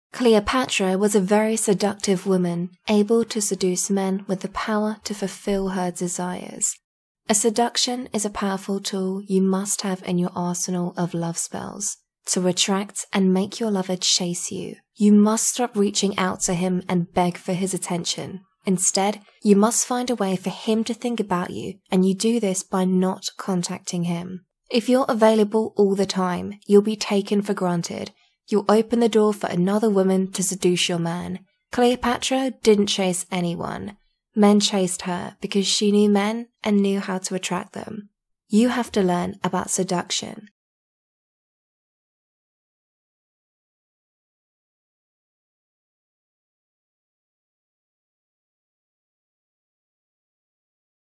Cleopatra's Love Spells That Work With Seduction
Love spells that work allow you to control your man's thoughts and behaviour, but also you must know how to cause attraction and seduction in your interactions with your lover.

Love spells can be ruined with negative emotions, arguments, and chasing him. If he's with another woman, you must awaken his desire for you and you can do that with a love spell that works to build attraction and passion between you and your man.